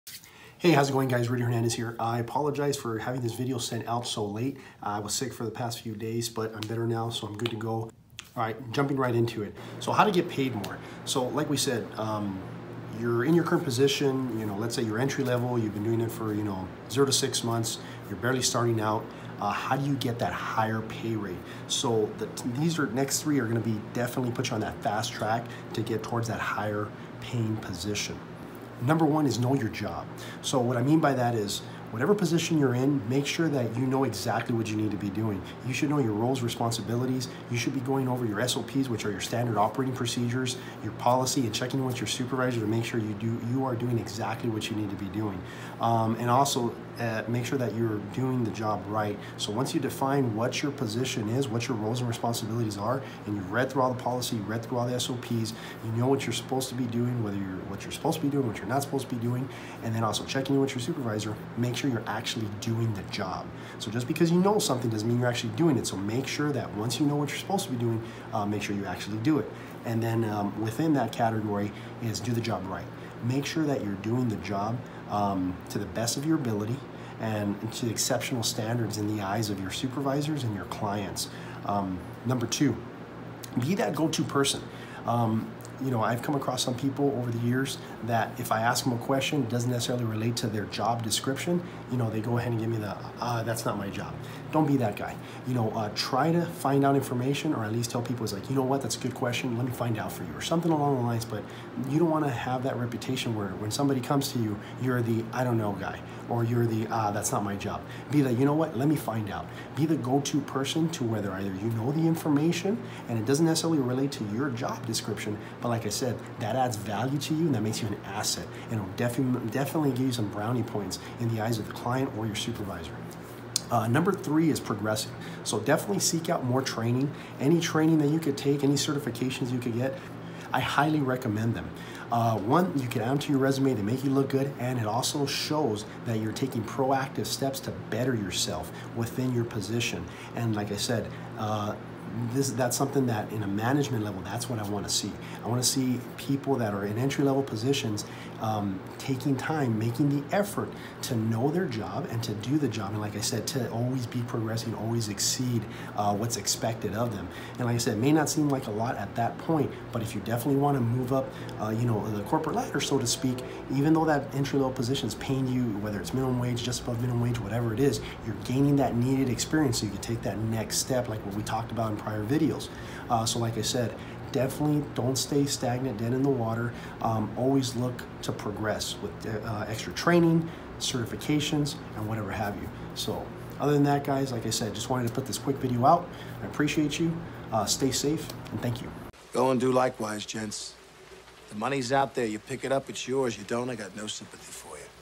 How to get paid more
In this video we briefly discuss steps to take that can help you get paid more or advance to a higher paying position.

These tips are from first hand experience and would benefit anyone in the security industry or any other industry for that matter.

1) Know your Job (00:29)

2) Be the "go-to" person (01:22)

3) Always be Progressing (02:04)

Stay safe!